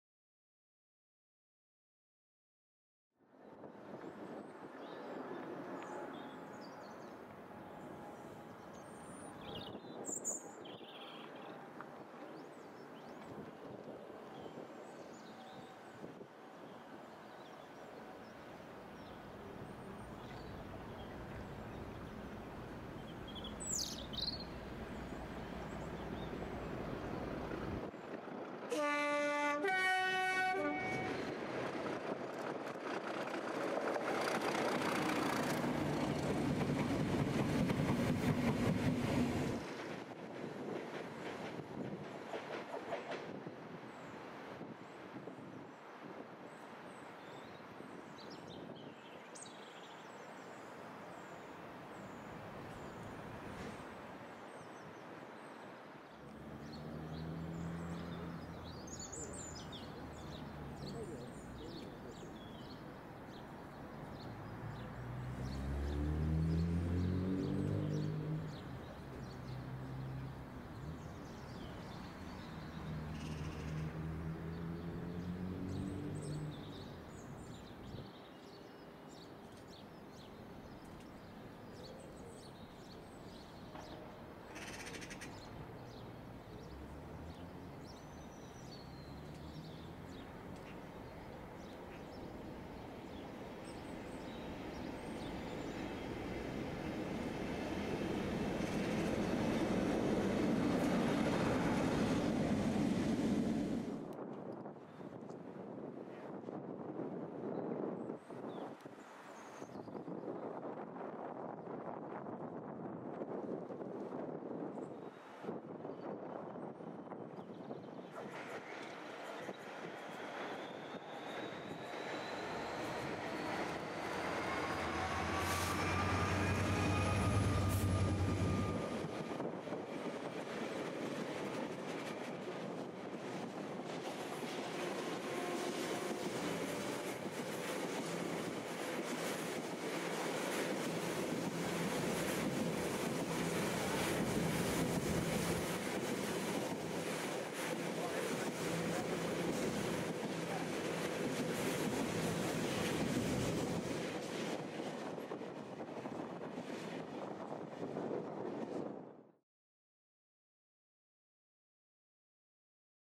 FINAL JOURNEY for 3 Class 91 Locos Headed for SCRAP - 47739 & Engineers Train @ Water Orton 03/02/23
A sad sight to see these InterCity 225 Locos being hauled away for scrap. See 37510 which is ironically a lot older than these locos hauling the 3 class 91's to Newport Sims through Water Orton.

We also see 47739 working light engine from Leicester LIP to Eastleigh and the engineering train from Bescot to Toton being hauled by 66778.